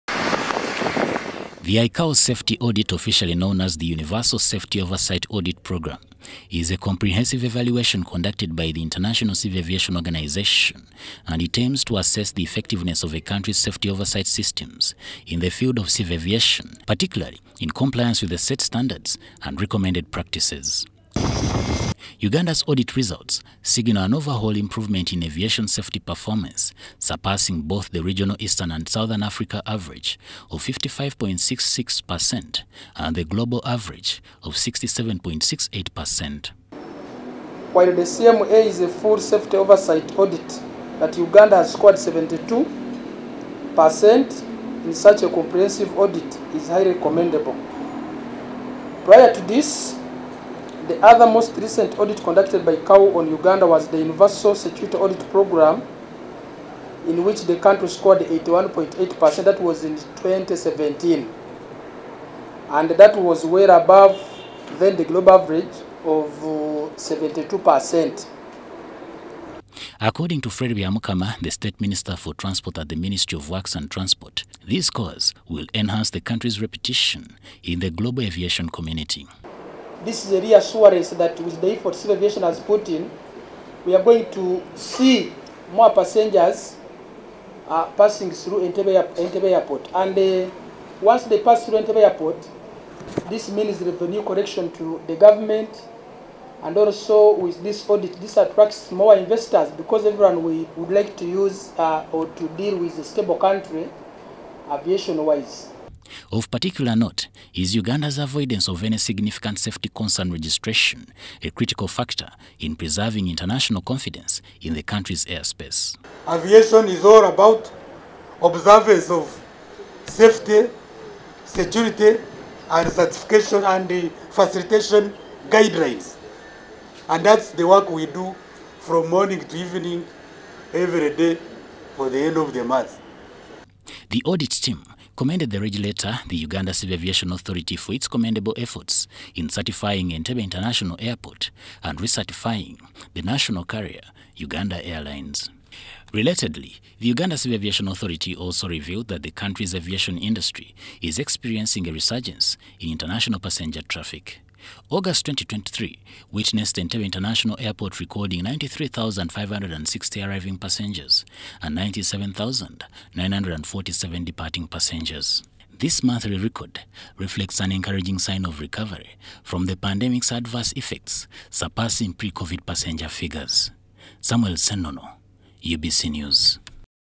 AVIATION MILESTONE- UGANDA SCORES 72.17% IN THE ICAO SAFETY OVERSIGHT AUDIT PROGRAM.
The International Civil Aviation Organisation has awarded Uganda an impressive score of 72.17% following a comprehensive Universal Safety Oversight Audit Programme. Conducted This  audit assessed the effectiveness of Uganda's safety oversight systems, aligning them with ICAO Standards and Recommended Practices.